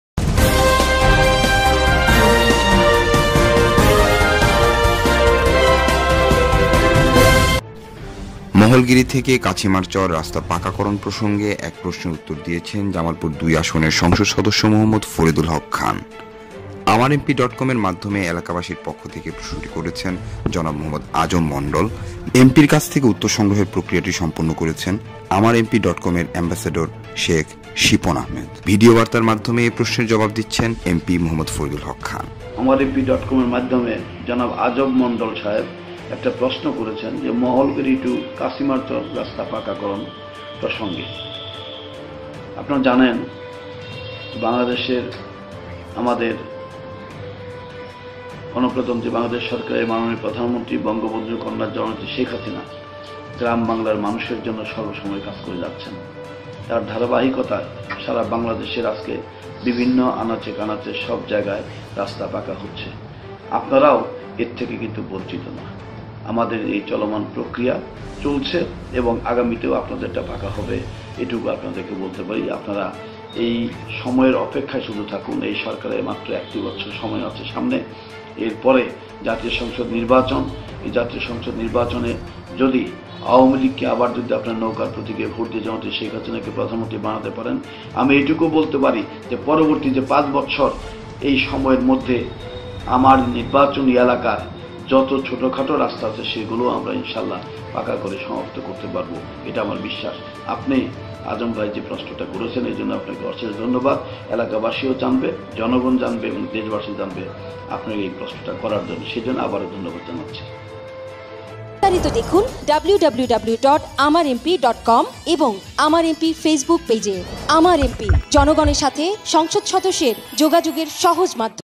Md. Faridul Haque Khan MP replied at AmarMP
মহলগিরী থেকে কাছিমারচর রাস্তা পাকাকরন প্রসঙ্গে এক প্রশ্নের উত্তর দিয়েছেন জামালপুর-২ আসনের সংসদ সদস্য মো: ফরিদুল হক খান। আমার এমপি ডট কমের মাধ্যমে এলাকাবাসীর পক্ষে থেকে প্রশ্নটি করেছেন জনাব মো: আজম মন্ডল।

এমপির কাছ থেকে উত্তর সংগ্রহের প্রক্রিয়াটি সম্পন্ন করেছেন আমার এমপি ডট কমের এম্বাসেডর শেখ শিপন আহম্মেদ।

ভিডিওবার্তার মাধ্যমে এ প্রশ্নের জবাব দিচ্ছেন এমপি মো: ফরিদুল হক খান।